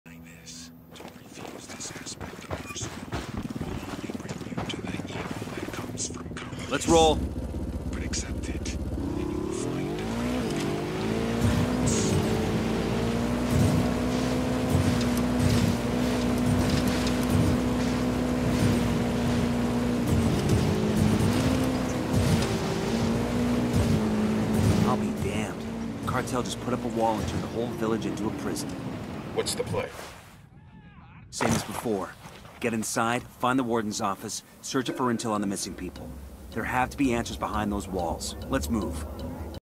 Fail Bike Jump Makes for a Shooting Star
What turn into an Epic Jump turned into a funny Superman Bug. Spoiler Alert. I didn't die. Jumped from a huge cliff and instead of crashing i flew over 400m through the air crash landed in the wall. Rolled through the barrier in the wall and i was perfectly fine.

This might be a funny Shooting Star Meme